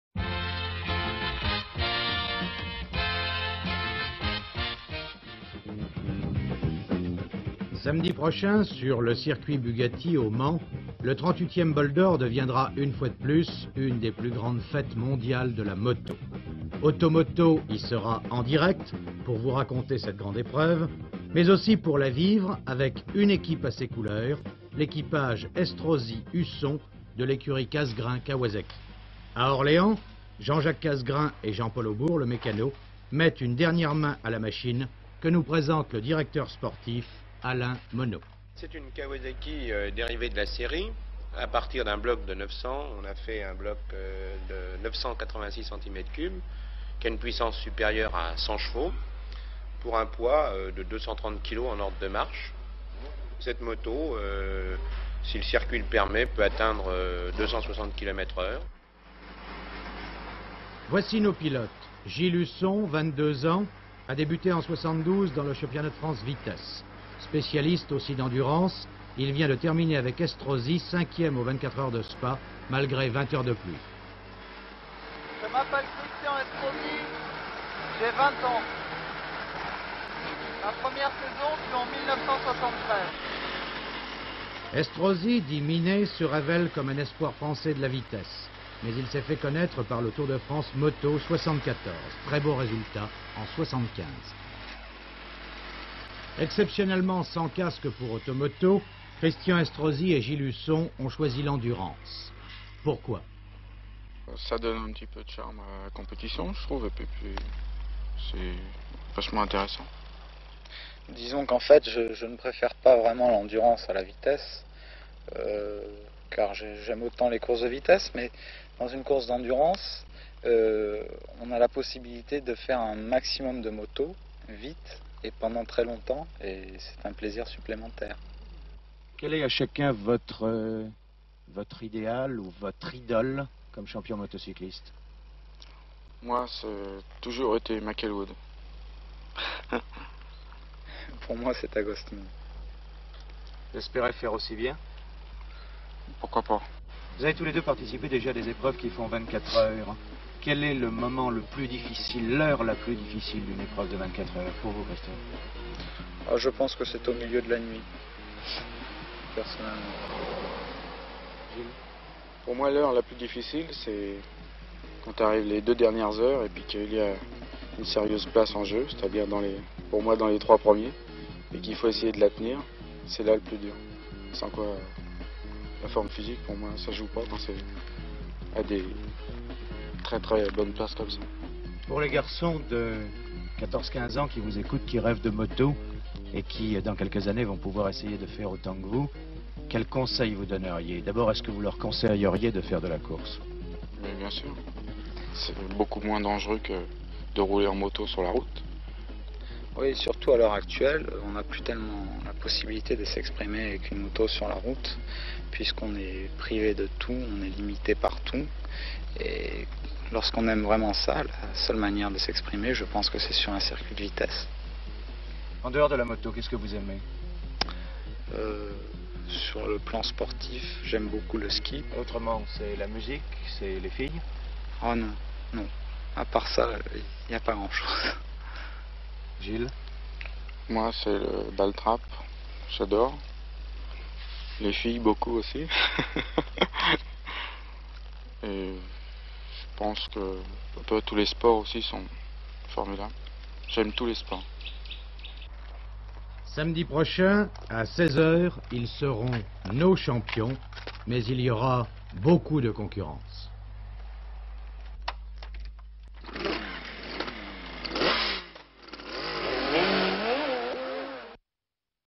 Christian Estrosi et l'amour de la moto, le bol d'or
Christian Estrosi à eu une folle jeunesse motocycliste. Dans ce reportage d'epoque decouvrez le maire de Nice comme jamais mais aussi le defunt Gilles Husson, ou encore Alain Monnot ect.. Une belle hisoire